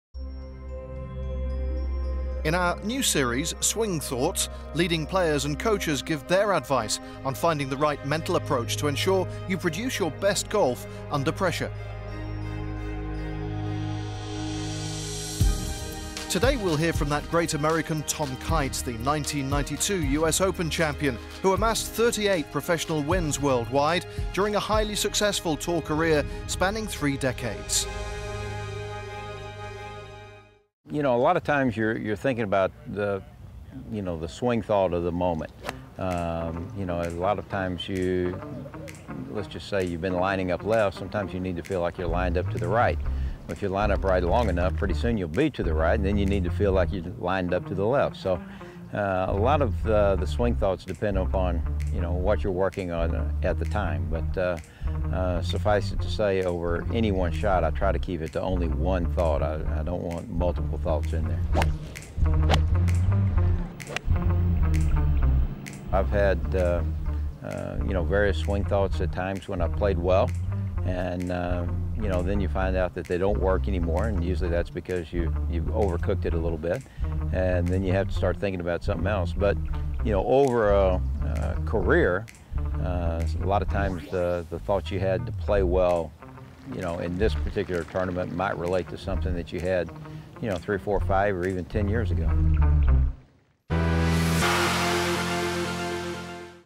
GW Swing Thoughts: Tom Kite
1992 U.S. Open champion and prolific PGA TOUR winner, Tom Kite, shares with us his take on swing thoughts